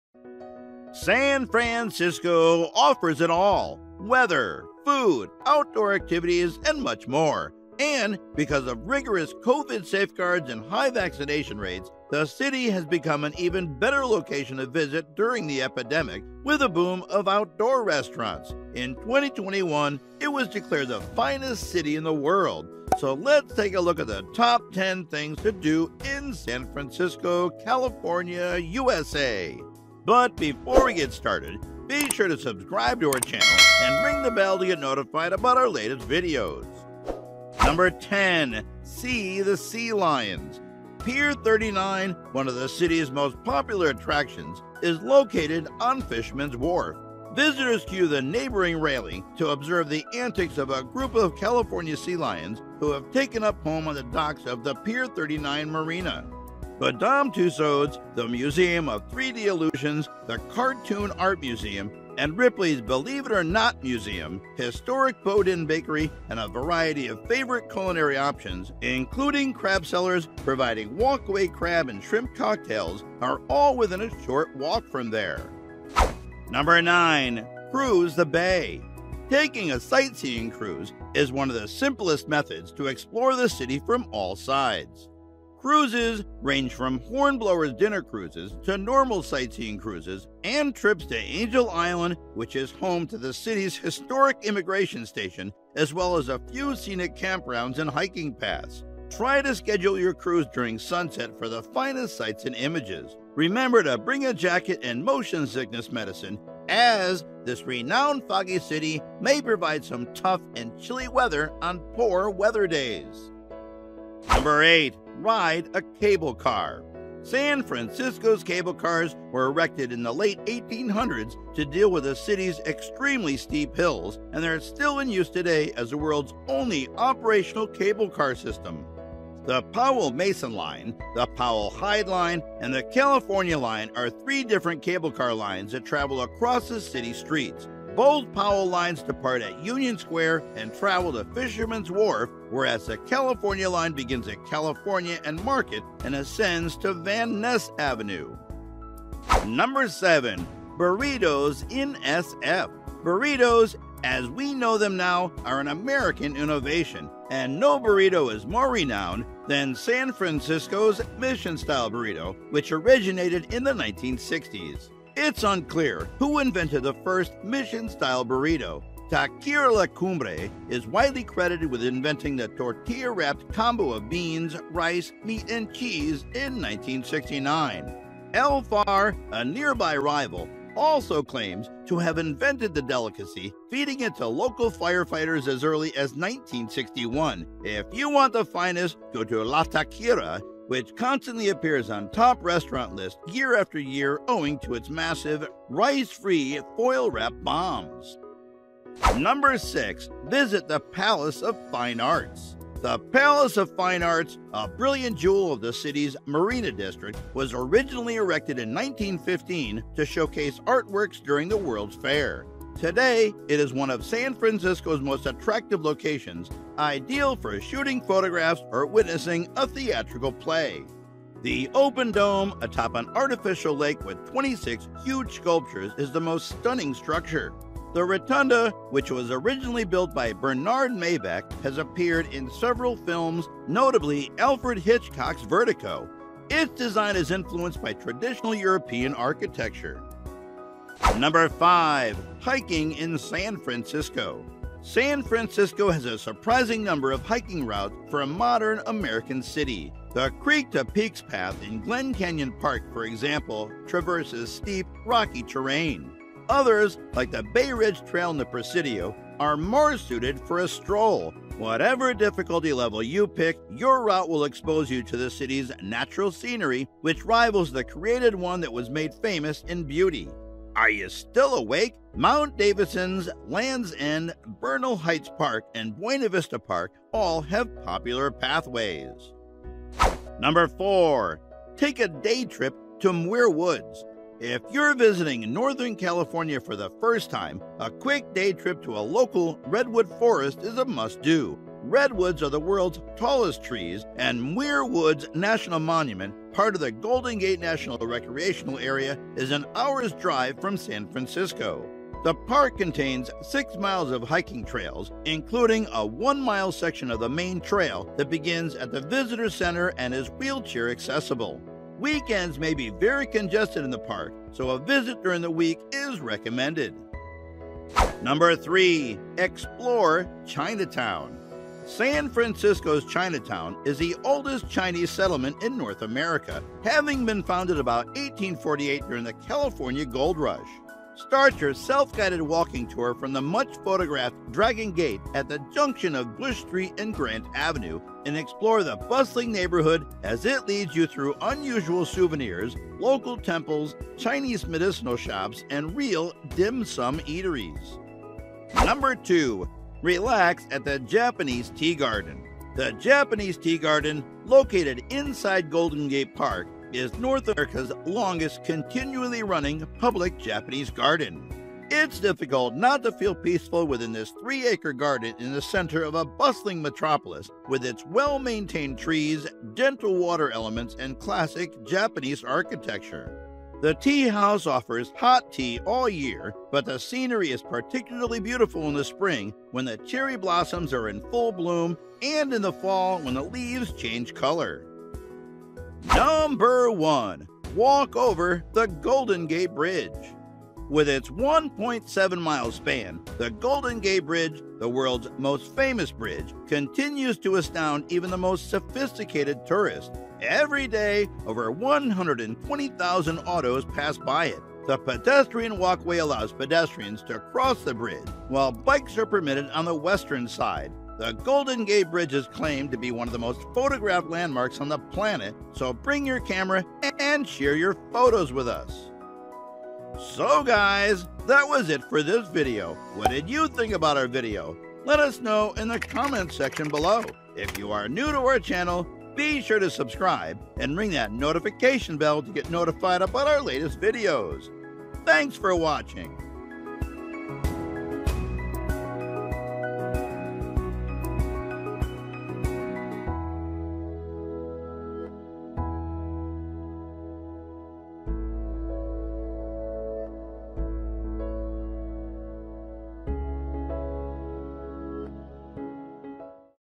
Top 10 Things to Do in San Francisco California USA
San Francisco offers it all: weather, food, outdoor activities, and much more. And, because of rigorous COVID safeguards and high vaccination rates; the city has become an even better location to visit during the epidemic, with a boom of outdoor restaurants. In 2021, it was declared the finest city in the world. So let’s take a look at the Top 10 Things to Do in San Francisco, California USA.

Note: some of the restaurants on my lists have closed due to current events, but have plans to re-open soon!